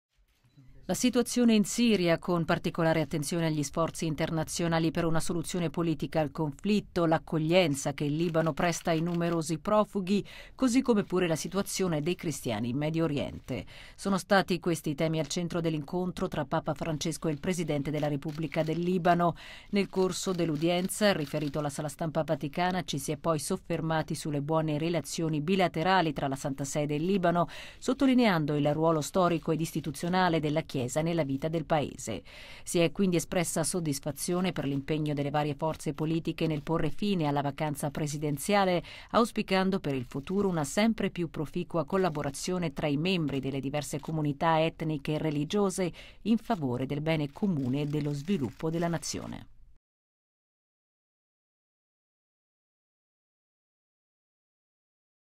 Udienza presidente Libano 16.03.2017
Udienza in Vaticano per Michel Aoun

Papa Francesco ha ricevuto il presidente della Repubblica del Libano, Michel Aoun, il quale successivamente ha incontrato il segretario di Stato, cardinale Pietro Parolin, accompagnato da mons. Paul Gallagher, segretario per i Rapporti con gli Stati.